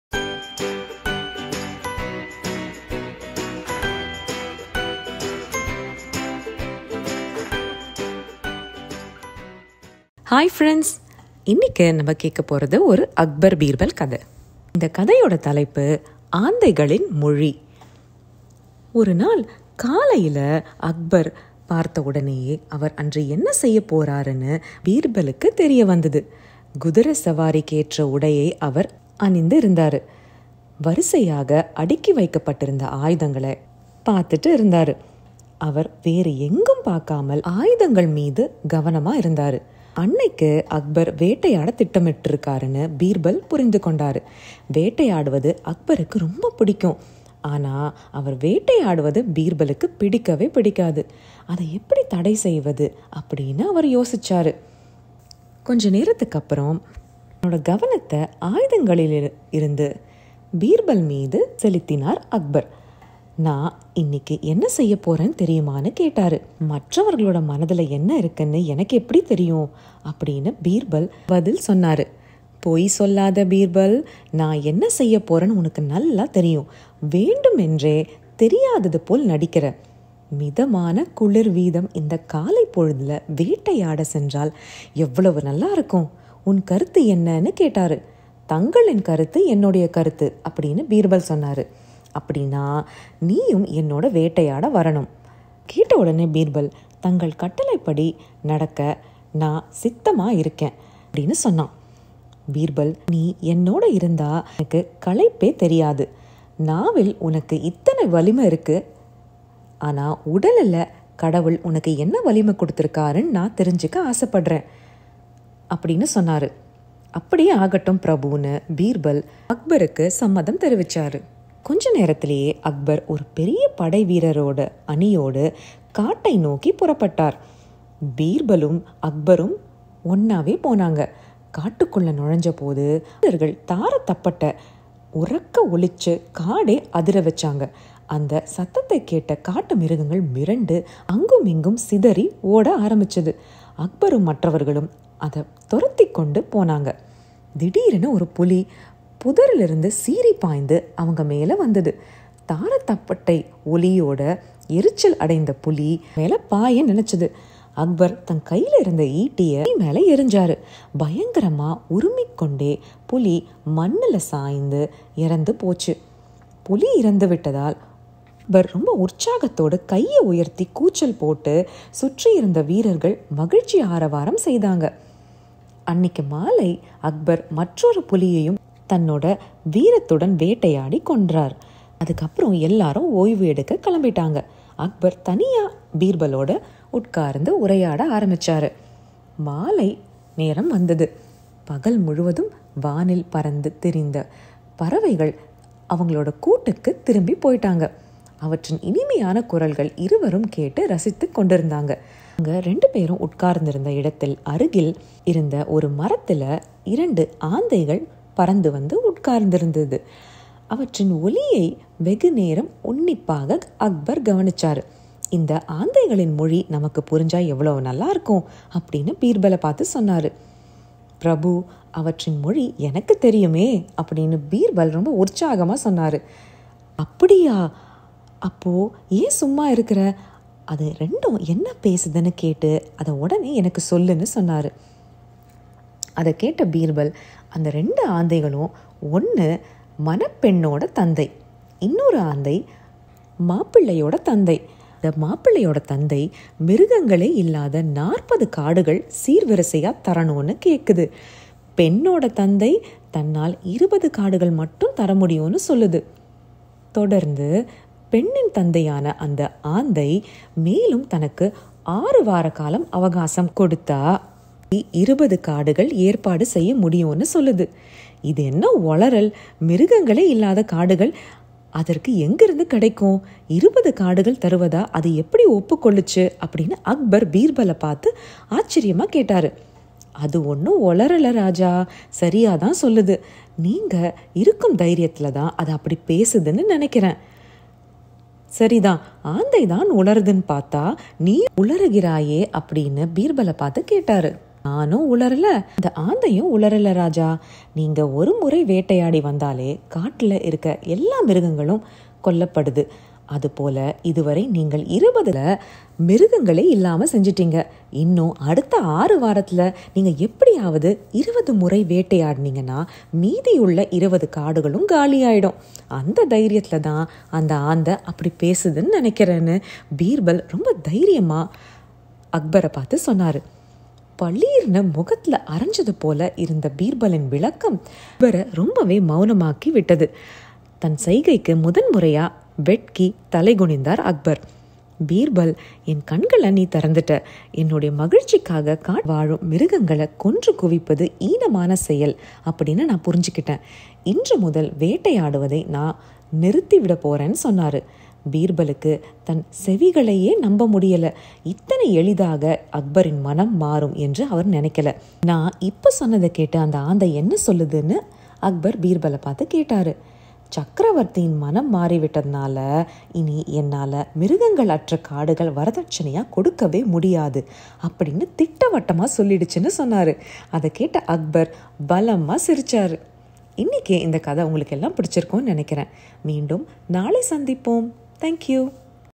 Aandhaigalin Mozhi/Akbar birbal series/Moralstories/Sirukadhaigal/Needhi kadhaigal/Bedtime stories
A story about Birbal teaching a lesson to Akbar without hurting his feelings.